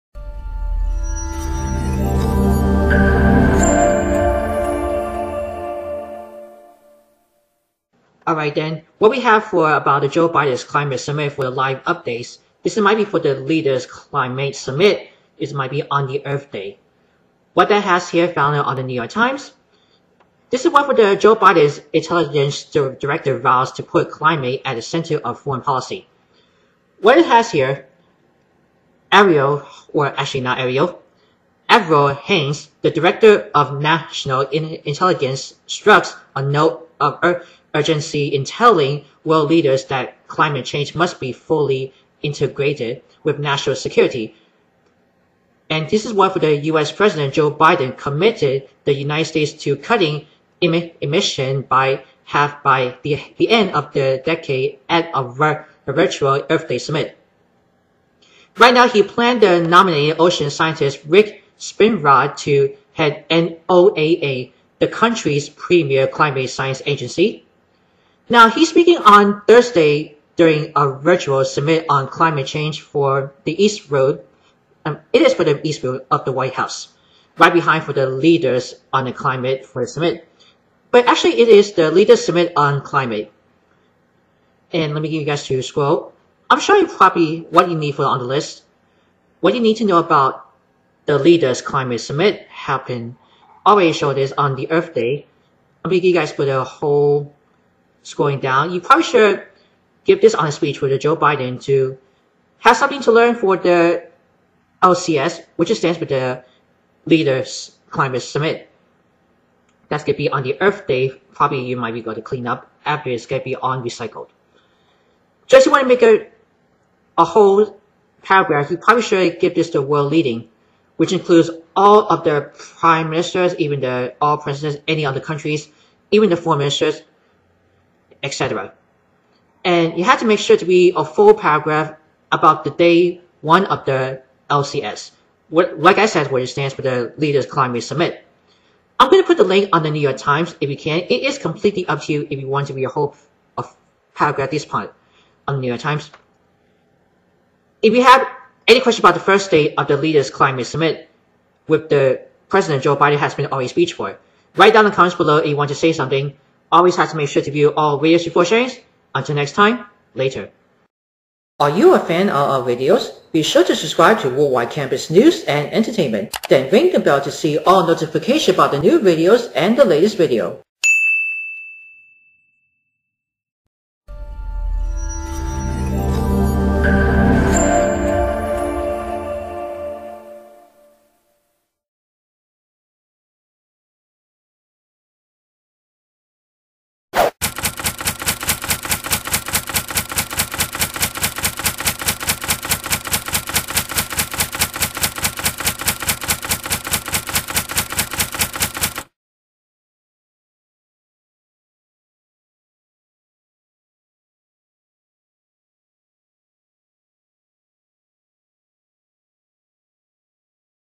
What’s on World Wide Campus? - Leaders Summit on Climate 2021 with Joe Biden
For this week on “What’s on World Wide Campus?”, here’s the update about Joe Biden’s Intelligence Director Vows to Put Climate at ‘Center’ of Foreign Policy was on Earth Day.

According to The New York Times, Avril Haines, the director of national intelligence, struck a note of urgency in telling world leaders that climate change must be “fully integrated” with national security. President Biden committed the United States to cutting emissions by half by the end of the decade at a virtual Earth Day summit.